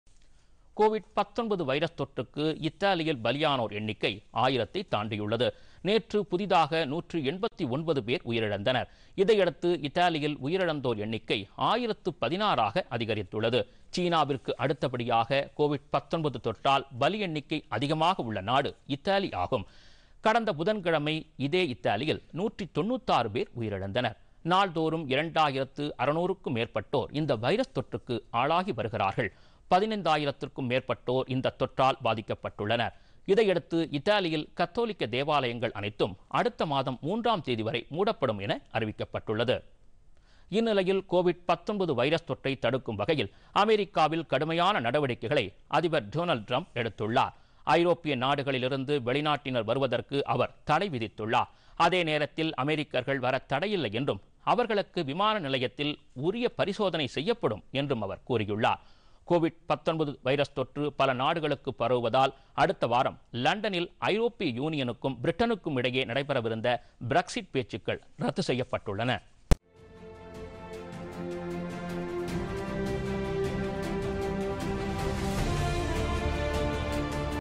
இத்தாலியில் பலி எண்ணிக்கை ஆயிரத்தை தாண்டியது
கோவிட்-19 வைரஸ் தொற்றால் இத்தாலியில் பலி எண்ணிக்கை ஆயிரத்தை தாண்டியது